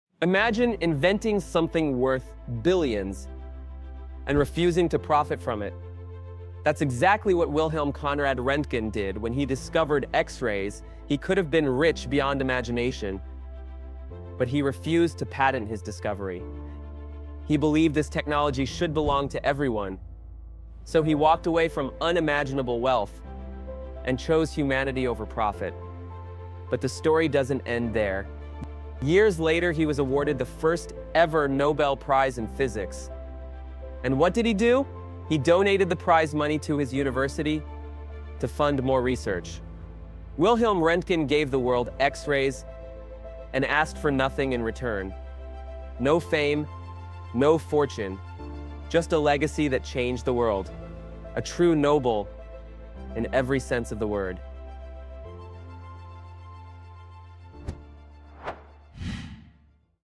The Man Who Chose Humanity Over Wealth 🌎
In 1895, Wilhelm Röntgen created the first human X-ray—and showed his wife’s skeleton hand.

A humble scientist changed the world—not with wealth or war, but with light.✨